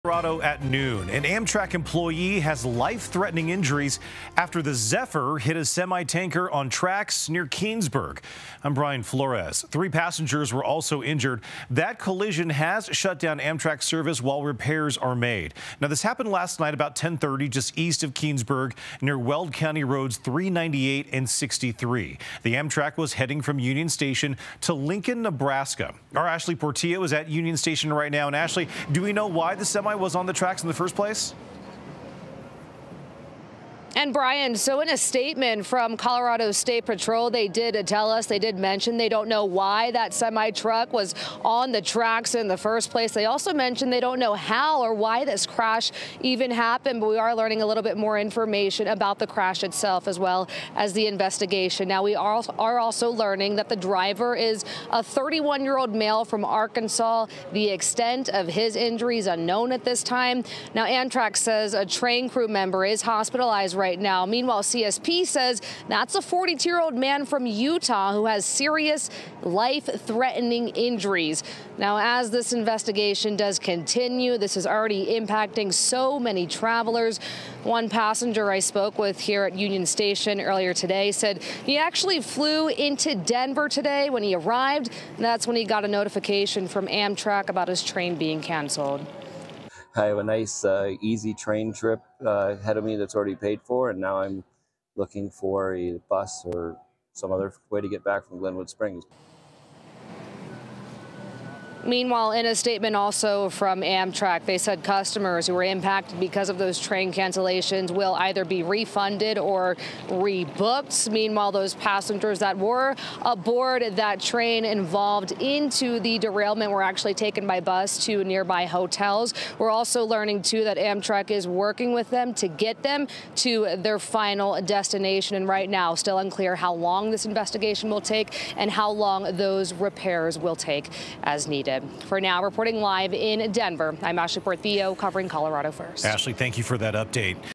3 injured when Amtrak train hits semi hauling milk, derails in Northern Colorado
An Amtrak train with dozens of people on board derailed after hitting a semi-truck hauling milk that was obstructing the tracks.